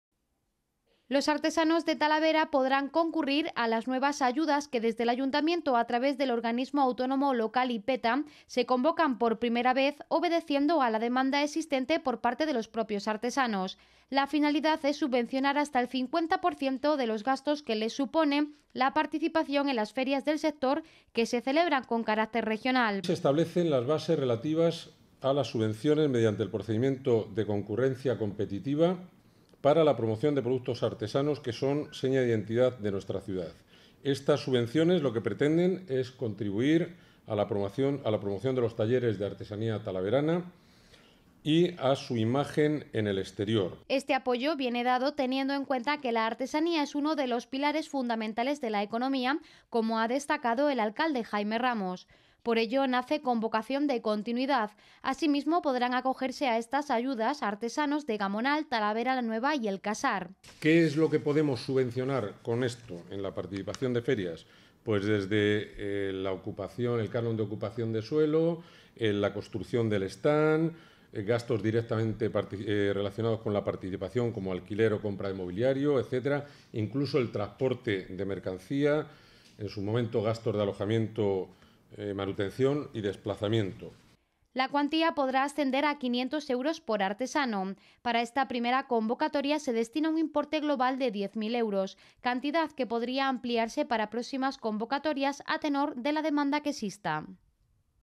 Ayudas para artesanos de Talavera
Los artesanos de Talavera contaran con una partida de diez mil euros para ayudas que pretenden contribuir a la promoción de los talleres artesanos  y el acceso de sus productos a nuevos mercados. Así ha sido anunciado esta mañana desde el consistorio talaverano.